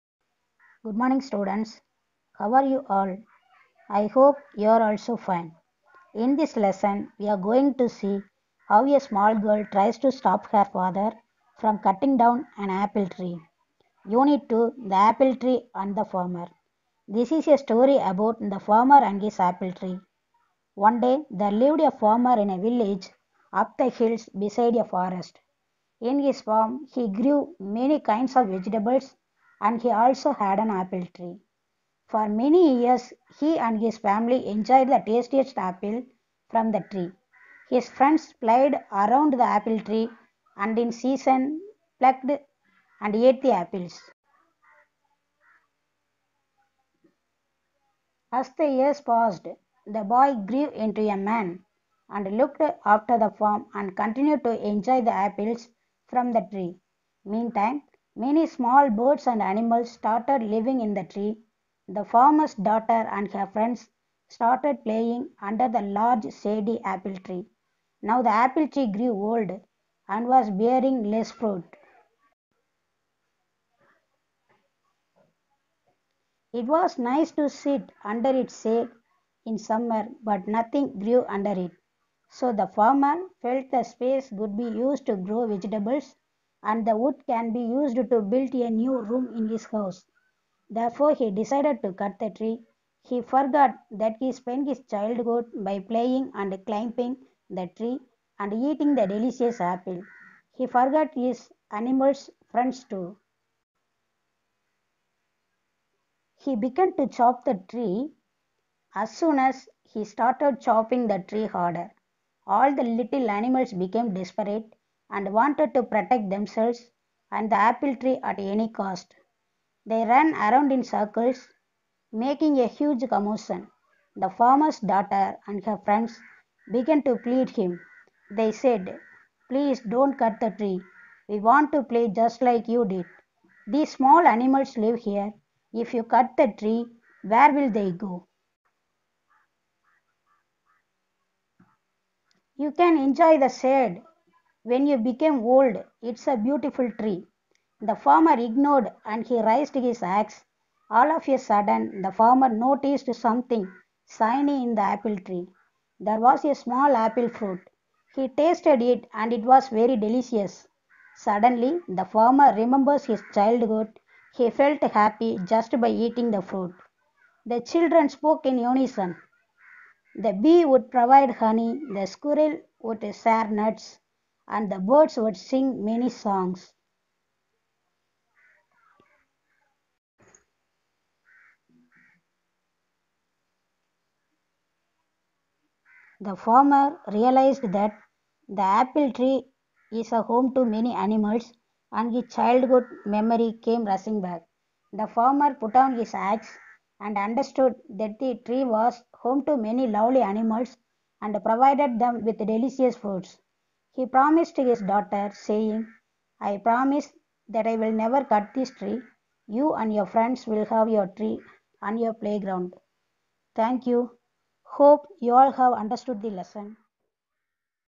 Unit- 2 The apple tree and the farmer 👍👍
Explanation of the story of The apple tree and the farmer